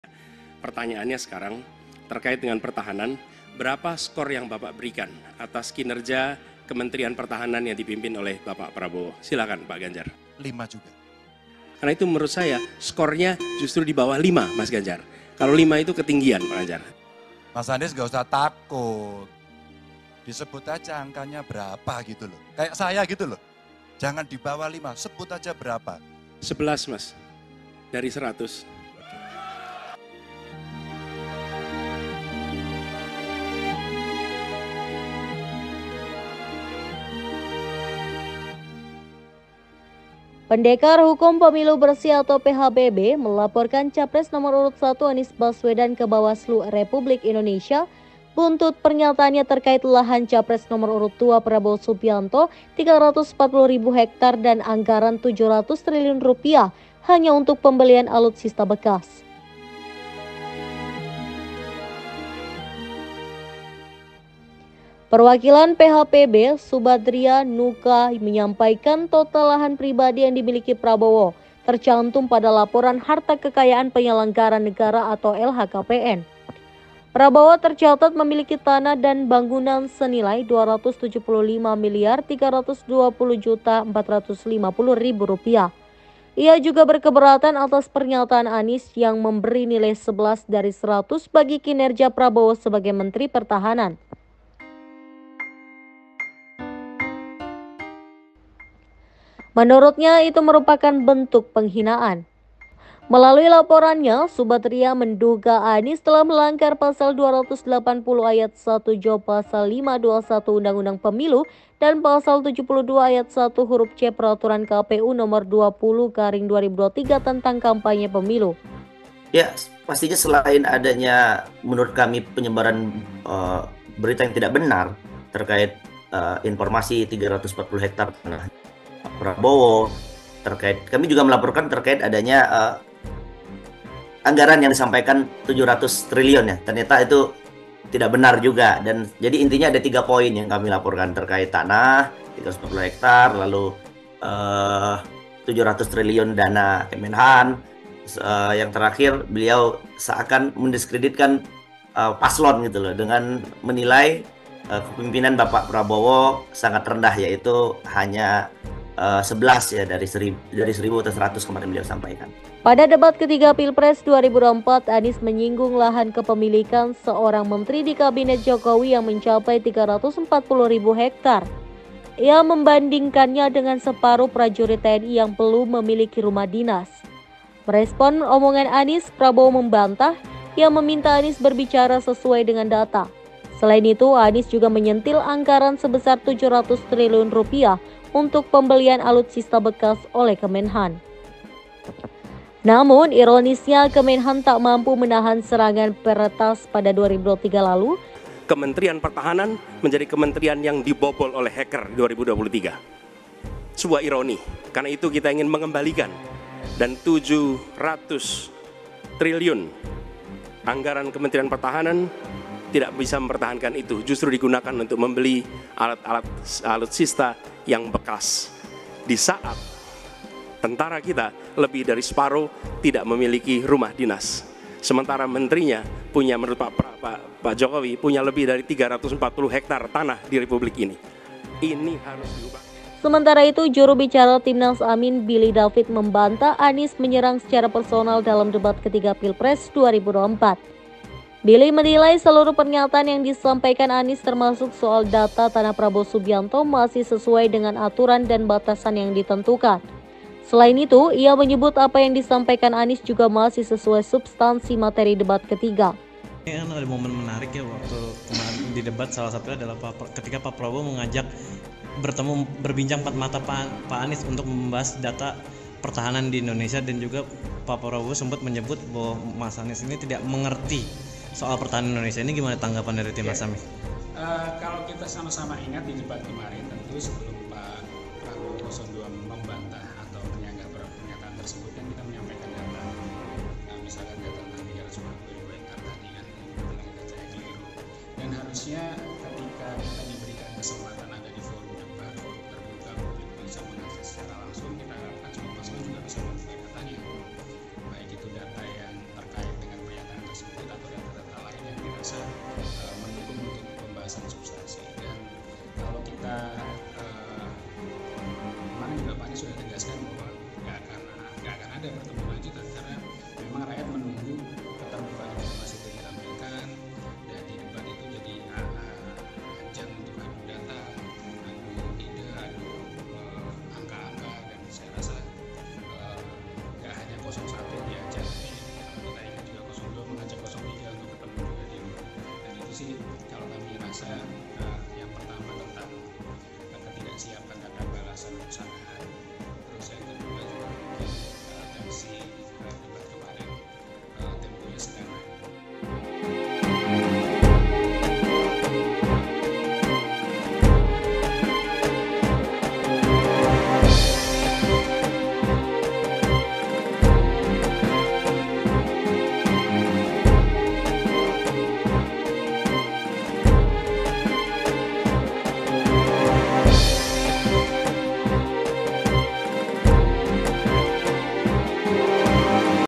Anies Baswedan Kembali Hadapi Gugatan, Dilaporkan ke Bawaslu Karena Serang Prabowo
Pendekar Hukum Pemilu Bersih (PHPB) melaporkan capres nomor urut 1, Anies Baswedan ke Bawaslu RI buntut pernyataannya terkait lahan capres nomor 2 Prabowo Subianto 340 ribu hektare dan anggaran Rp700 triliun hanya untuk pembelian alutsista bekas.

Perwakilan PHPB, Subadria Nuka menyampaikan total lahan pribadi yang dimiliki Prabowo tercantum pada laporan harta kekayaan penyelenggara negara (LHKPN).

Prabowo tercatat memiliki tanah dan bangunan senilai Rp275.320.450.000.

Ia juga berkeberatan atas pernyataan Anies yang memberi nilai 11 dari 100 bagi kinerja Prabowo sebagai Menteri Pertahanan.

Menurutnya, itu merupakan bentuk penghinaan.

Melalui laporannya, Subadria menduga Anies telah melanggar Pasal 280 ayat (1) jo Pasal 521 UU Pemilu dan Pasal 72 ayat (1) huruf c Peraturan KPU Nomor 20/2023 tentang Kampanye Pemilu.

"Karena diketahui jumlah anggaran Kemenhan tidak mencapai Rp700 Triliun dan terkait bidang-bidang tanah yang dimiliki oleh capres nomor urut 2 Prabowo Subianto adalah seluas 340 (ribu) hektare, maka hal tersebut adalah tidak benar" Perwakilan PHPB, Subadria Nuka, dikutip dari CNN.

Pada debat ketiga Pilpres 2024, Anies menyinggung lahan kepemilikan seorang menteri di Kabinet Jokowi yang mencapai 340 ribu hektare.

Ia membandingkannya dengan separuh prajurit TNI yang belum memiliki rumah dinas.

Merespons omongan Anies, Prabowo membantah. Ia meminta Anies berbicara sesuai dengan data.

Selain itu, Anies juga menyentil anggaran sebesar Rp700 triliun untuk pembelian alutsista bekas oleh Kemenhan.

Namun, ironisnya Kemenhan tak mampu menahan serangan peretas pada 2023 lalu.

Sementara itu, Juru Bicara Timnas AMIN Billy David membantah Anies menyerang secara personal dalam debat ketiga Pilpres 2024.

Billy menilai seluruh pernyataan yang disampaikan Anies termasuk soal data tanah Prabowo Subianto masih sesuai dengan aturan dan batasan yang ditentukan.

Selain itu, ia menyebut apa yang disampaikan Anies juga masih sesuai substansi materi debat ketiga.

"Apa yang disampaikan Pak Anies saya rasa dalam batasan dan kaidah yang tepat, dan semua yang disampaikan juga saya rasa cukup substansial meskipun belum semua tersampaikan" Juru Bicara Timnas AMIN Billy David, dikutip dari CNN.